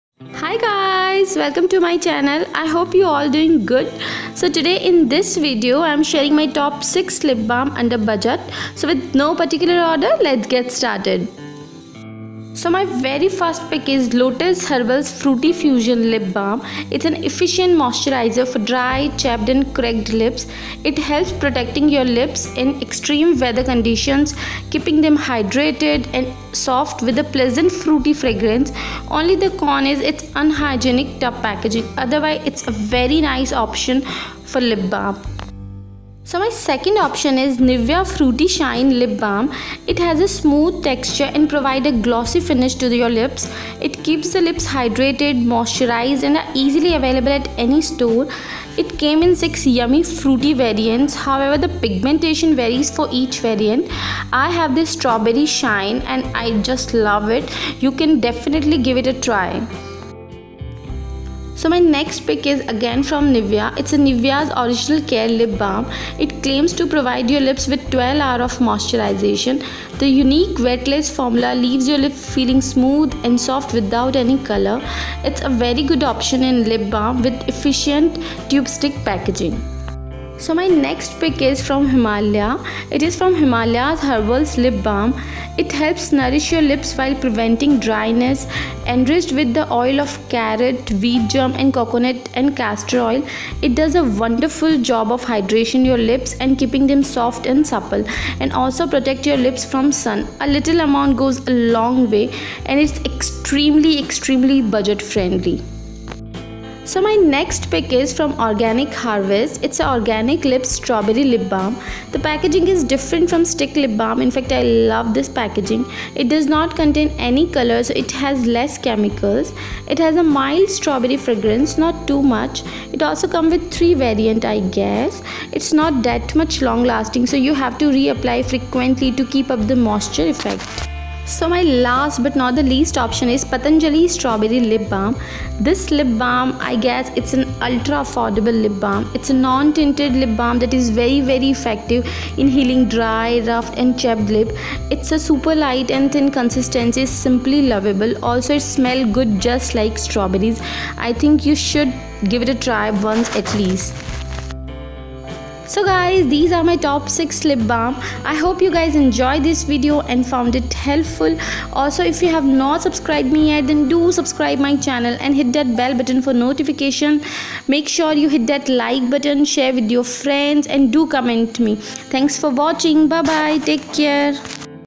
Top 6 Lip Balm Under Rs.200 | Affordable & Effective | Budget Beauty Series
Hello everyone :)

As title suggest, you guys already know that it's a new video in budget beauty series.
As monsoon start to begin, lip balm plays an important role in our beauty regime. We can't ignore importance of lip balm because we all love soft, supple & baby pink lips.

So I thought to share with you all my top 6 Lip balm under budget but yet very effective.

I hope you guys love & enjoy this video, also found in helpful.

I would love to read all your feedback.

(It's free of cost)

Instagram as kabra_vijayaa21
Twitter as kabra_vijayaa21

Spread Positivity & Love
Vijayaa